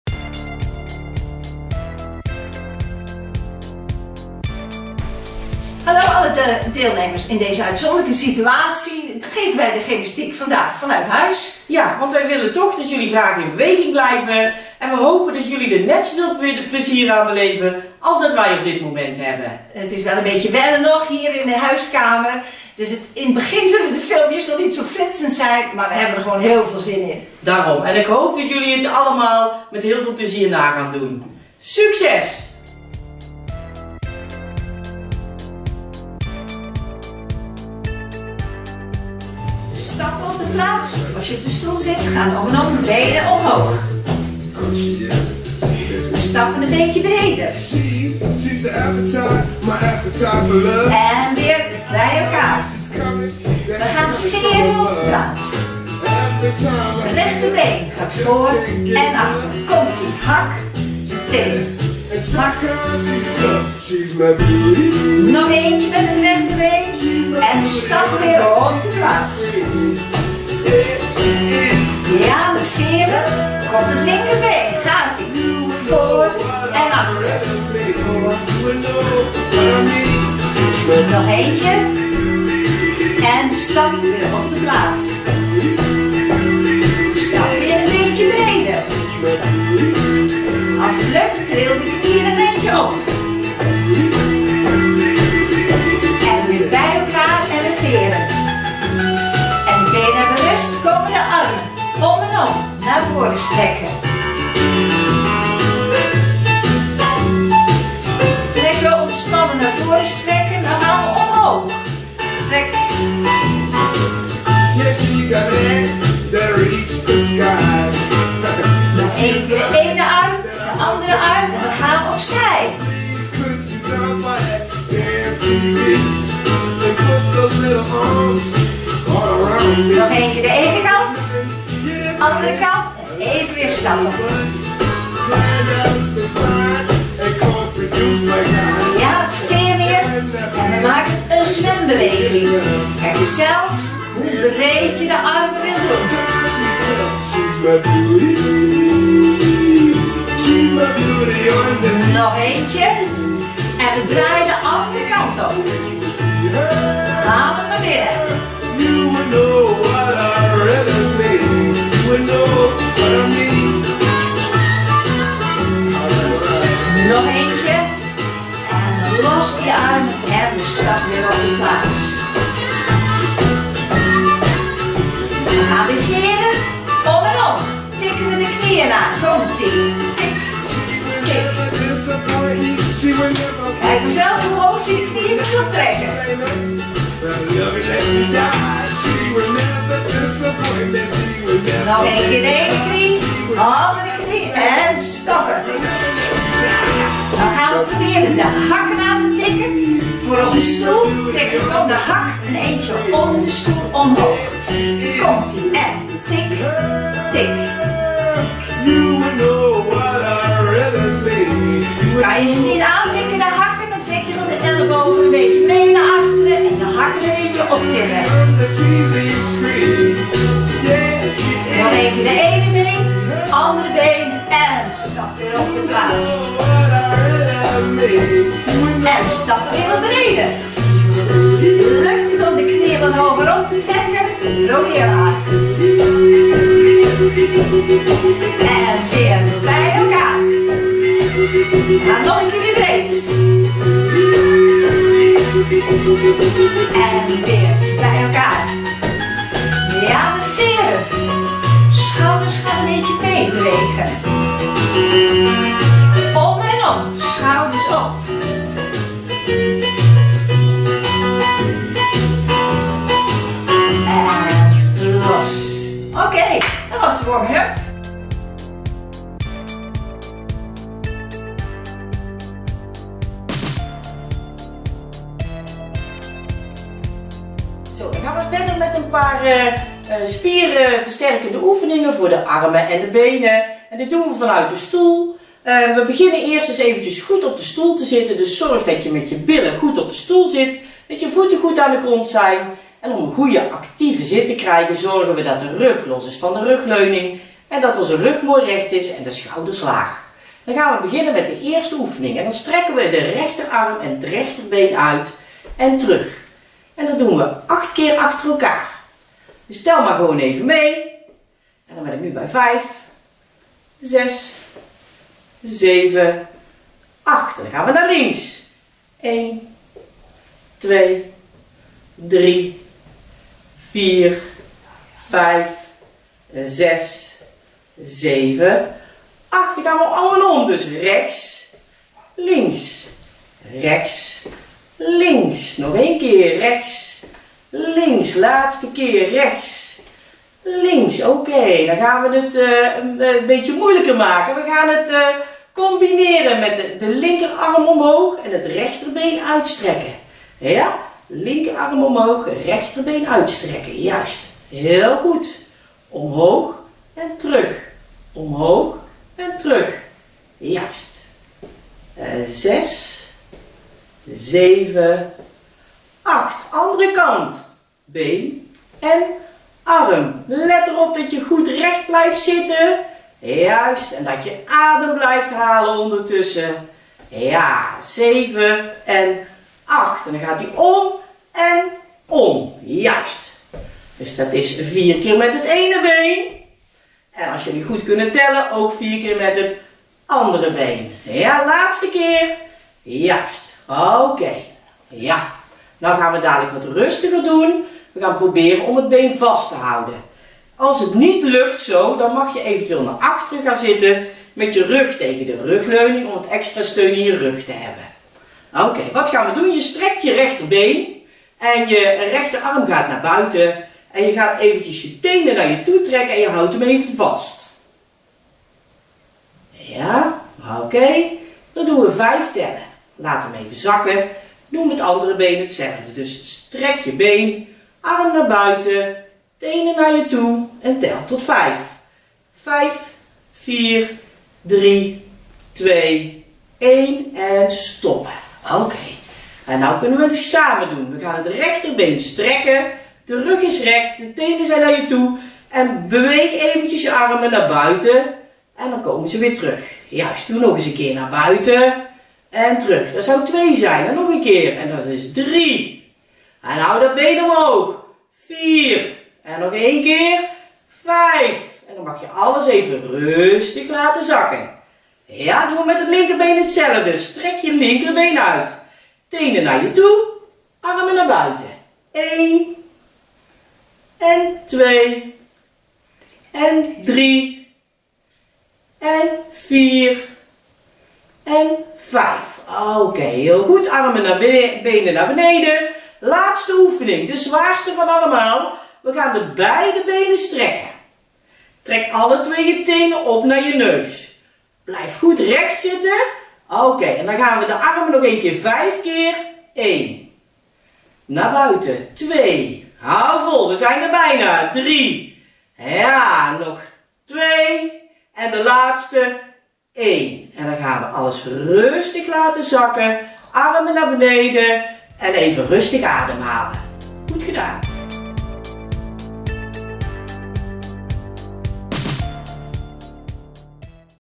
'S-PORT - Beweeg u Fit! Gym: Les 1
Doe thuis mee met Beweeg u fit! In les 1 hebben Mirjam en Lisanne een korte les opgenomen. Doe mee en blijf in conditie!